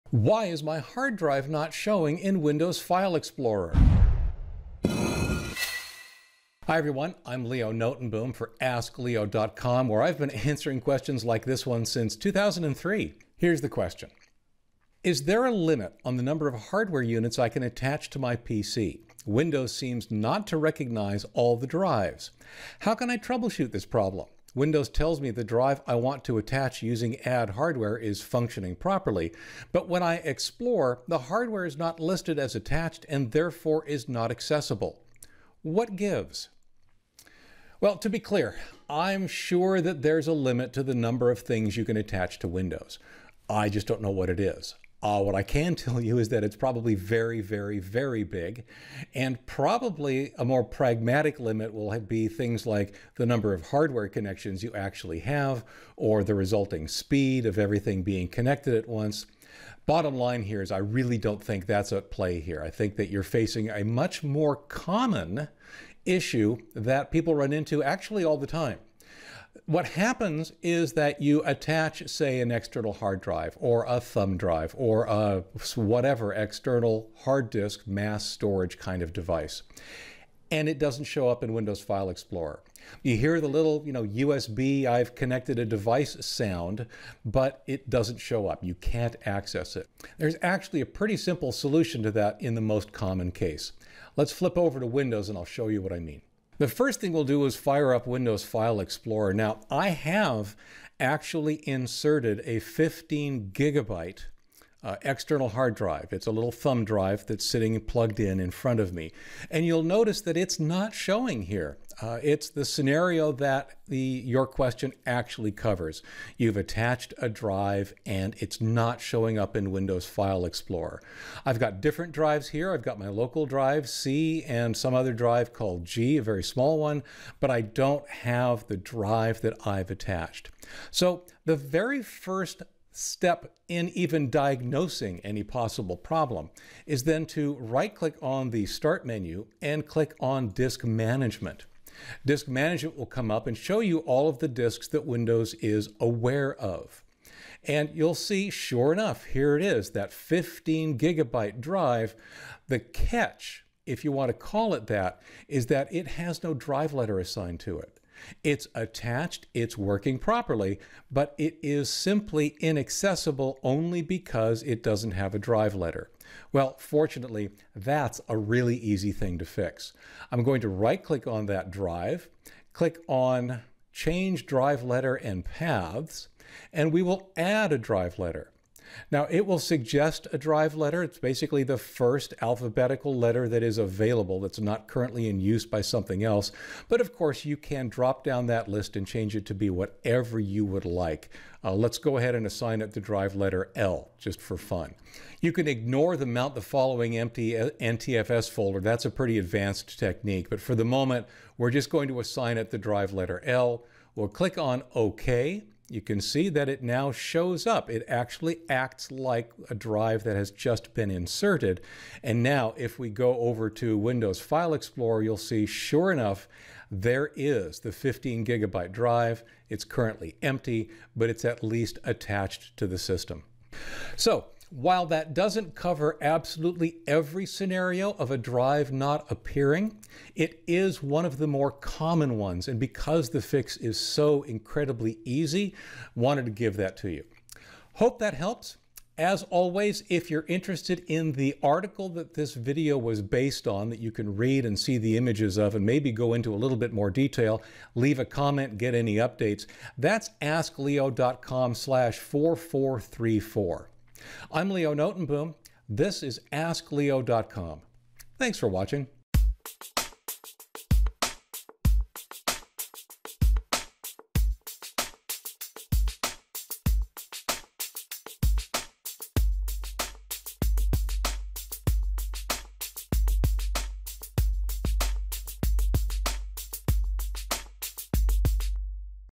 Why Is My Hard Drive Not Showing in Windows File Explorer?
❗❓  Occasionally, attaching a drive to a computer will appear to work, except the drive's nowhere to be found. I'll walk through one common solution.